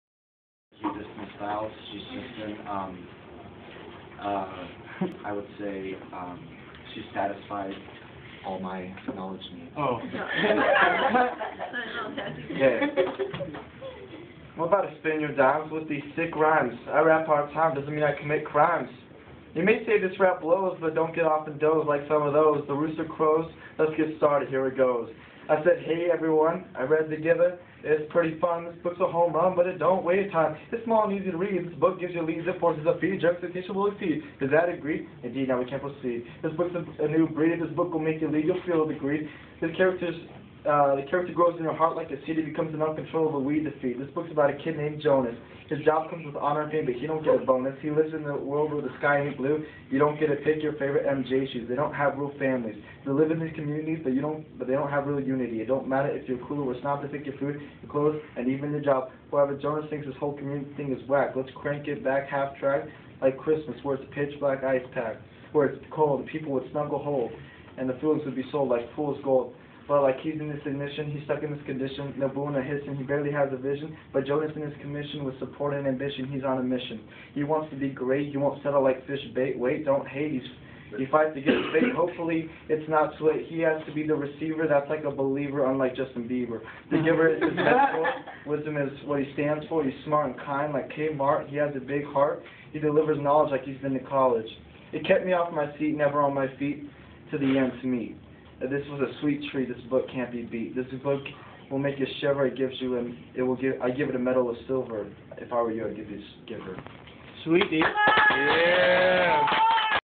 Austin Molt- A rap about The Giver
Austin raps in English class